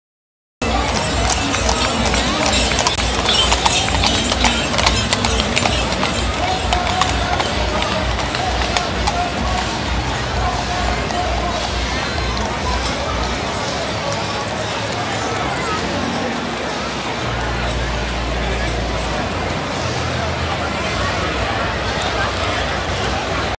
Seville Feria, April Fair 세비야 4월의 축제
Seville Feria is a week of dancing, dringking, eating, and socialising, some going all night in April. The city is filled with women in fabulous dresses, and men in horse riding suits. It takes place in the "Real de la Feria," which is next to an amusement park. almost a thousand tents fill the area, which serves foods and drinks, where families hang out and dance. Due to the hundreds of horses that transports these merry partygoers, the city is filled with the smell of manuer. Yet, the colors and the excitement makes it a worthwhile stay during this once in a year event!

세비아의 4월의 축제는 봄의 축제라는 별명으로 알려져있습니다. 1주일간 여성들은 화려한 플라멩코 드레스를 입고 남성들은 정장이나 기마 슈트를 빼입고 "레알 데 레 페라"라는 장소를 향해 갑니다. 이 곳에서는 1000개 가까이 되는 텐트를 펼치고 음식점들이 준비되어있습니다. 가족들, 연인들은 이 곳에서 음식과 술을 즐기며 밤새도록 놀기도 합니다. 단점이라면 운송의 수단인 말들이 도시를 누비는데 똥냄새가 심하여 걸어다닌다면 강가를 따라 다니라고 조언해드리고 싶습니다.